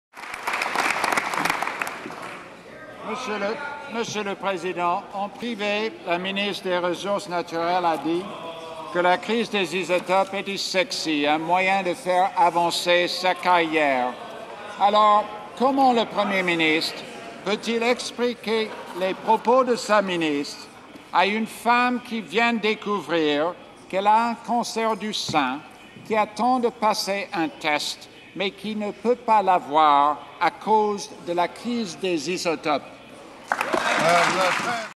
Michael Ignatieff demande à la ministre Raitt de s'expliquer pour ses propos déplacé
Michael Ignatieff demande à la ministre Raitt de s'expliquer pour ses propos déplacé sur la crise des isotopes.